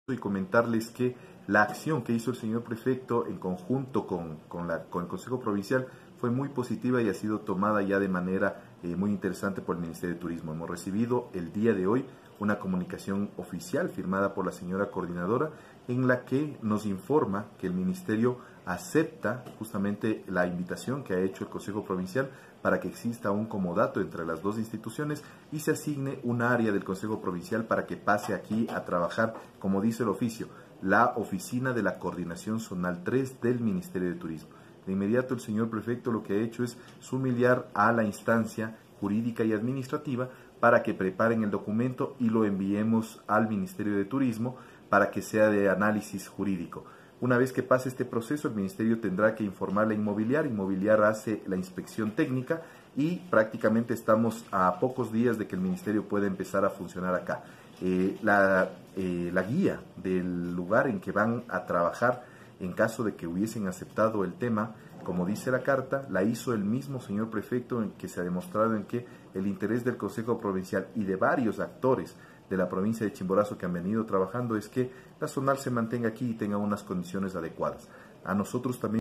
¿Qué pasó con el ofrecimiento del Gobierno provincial de Chimborazo de entregar instalaciones
¿Qué pasó con el ofrecimiento del Gobierno provincial de Chimborazo de entregar instalaciones para el funcionamiento de la Coordinación Zonal de Turismo? Luis Xavier Falconí, asesor de Turismo del Consejo Provincial, explica.